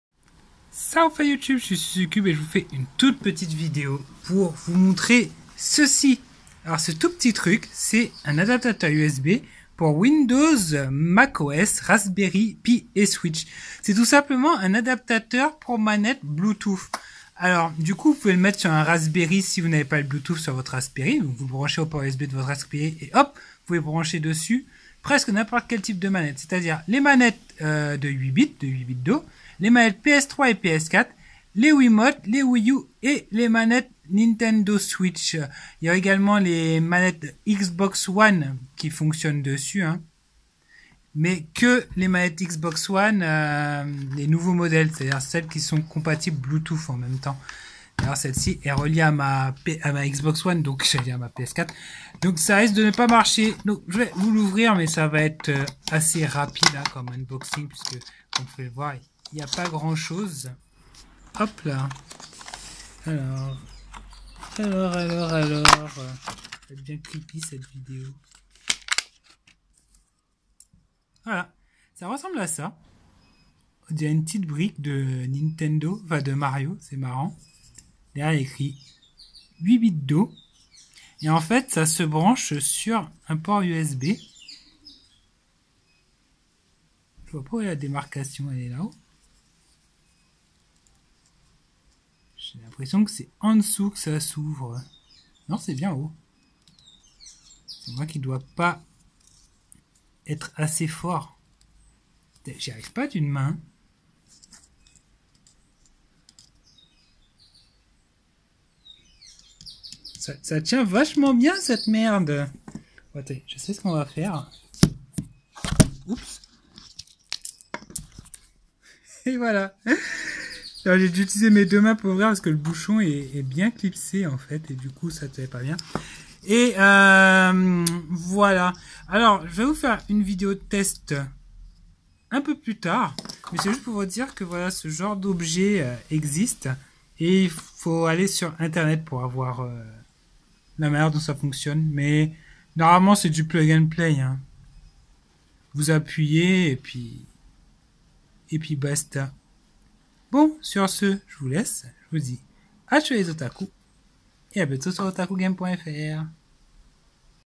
📦 Unboxing du 8bitdo USB Wireless Adapter
Petit unboxing rapide de la clé USB vous permettant de jouer à la Nintendo Switch avec vos manettes PS4 ou Xbox One ! C'est complétement dingue et c'est fait par 8bitdo !